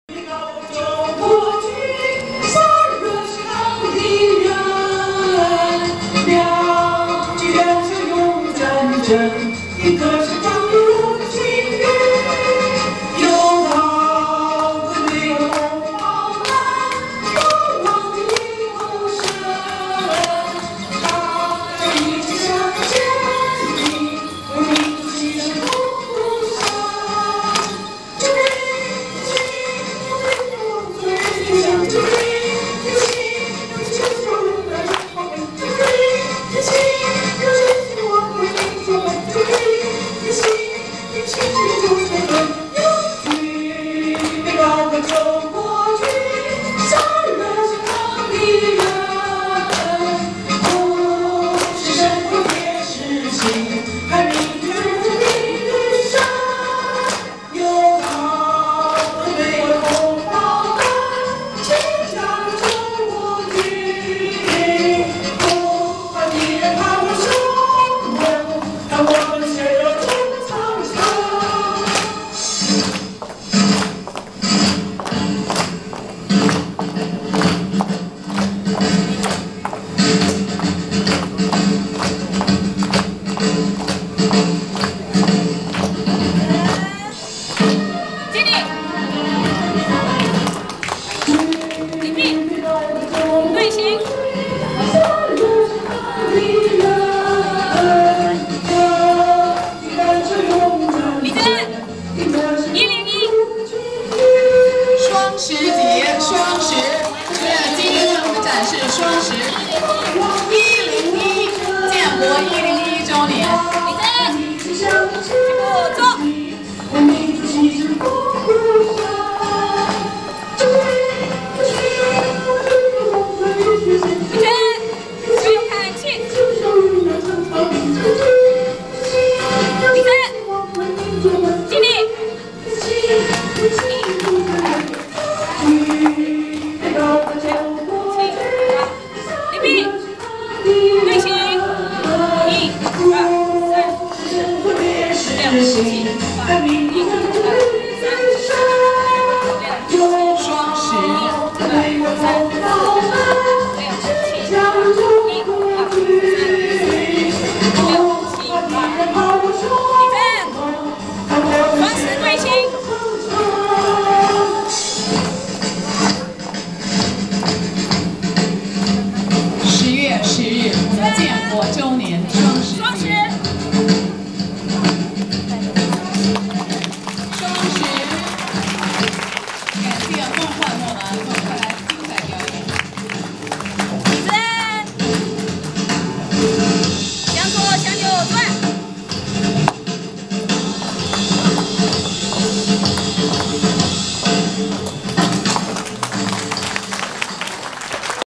10/20/2012 TBALV 杯酒高歌
Flag Raising and Celebration Ceremony
                 Taiwan Benevolent Association of Las Vegas
Hosted by:
                 International Culture Connection Club
Shirley Liu, Krystal Lee,Annie Ting,Pao Chin Cheng, Lily Mead, Sandy Western